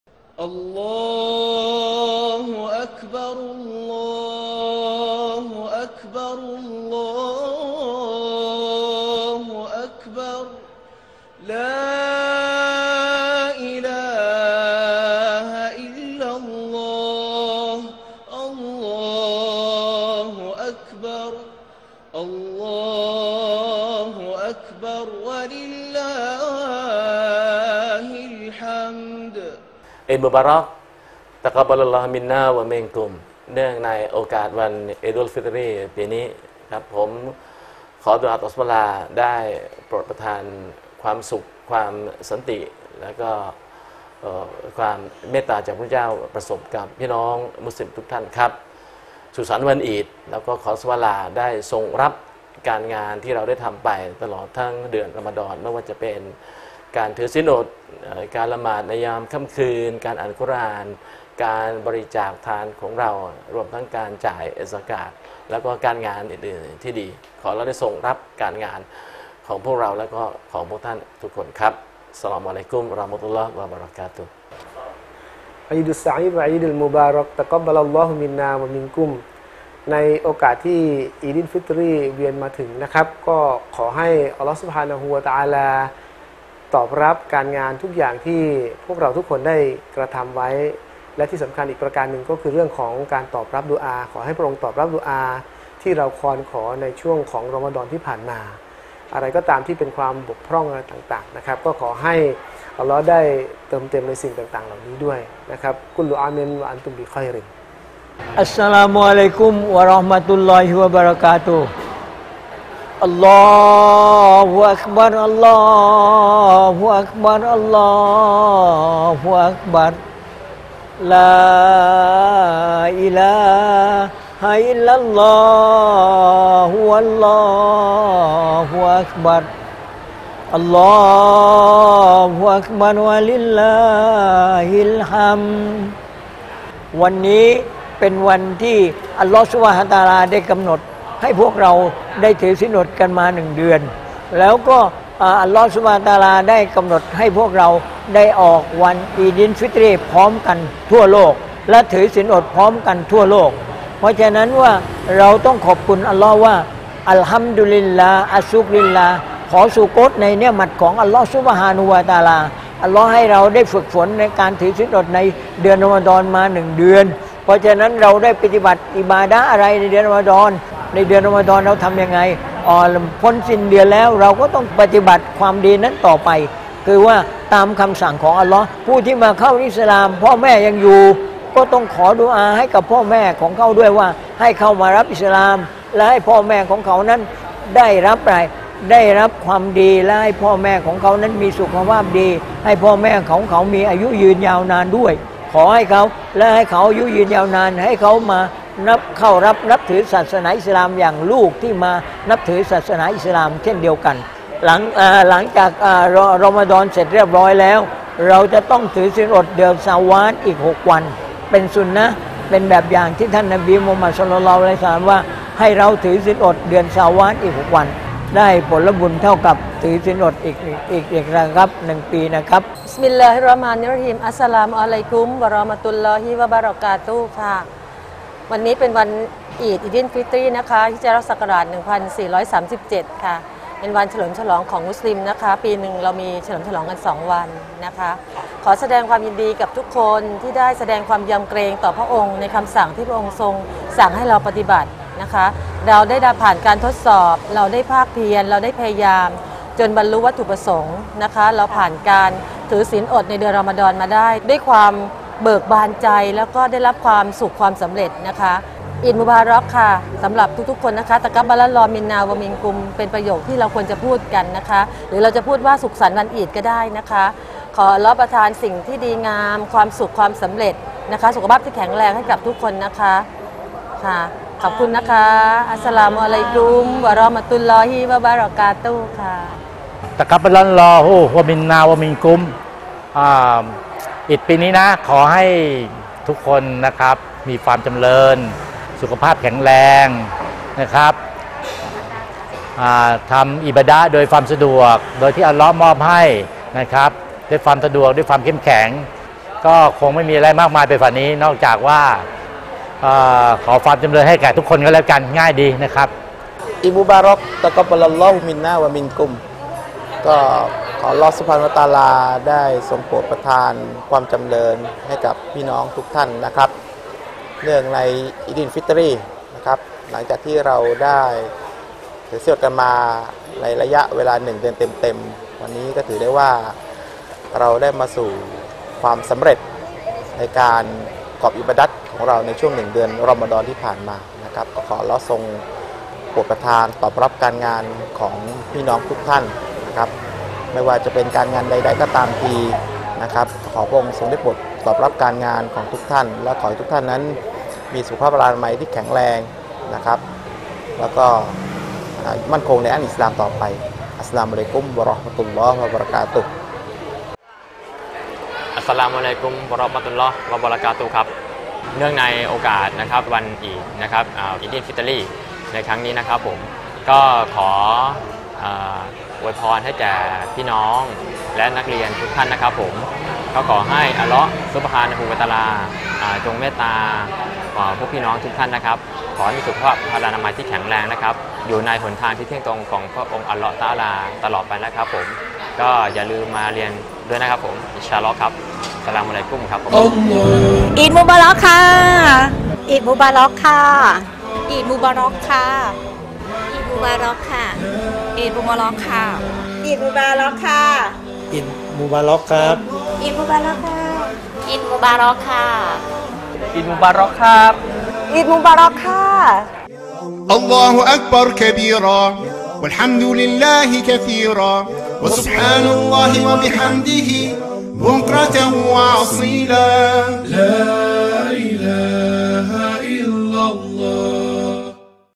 อวยพร เนื่องในวันอีดี้ลฟิตรี่ ฮ.ศ.1437
อวยพรวันอีดี้ลฟิตรี่ ฮ.ศ.1437
จากบรรดาคณาจารย์ และเจ้าหน้าที่
โครงการอบรมผู้สนใจอิสลาม มูลนิธิสันติชน

สนใจศึกษาอิสลาม ติดต่อ

ดีวีดีการบรรยายทั้งหมดมีจำหน่ายที่โครงกา­รอบรมผู้สนใจอิสลาม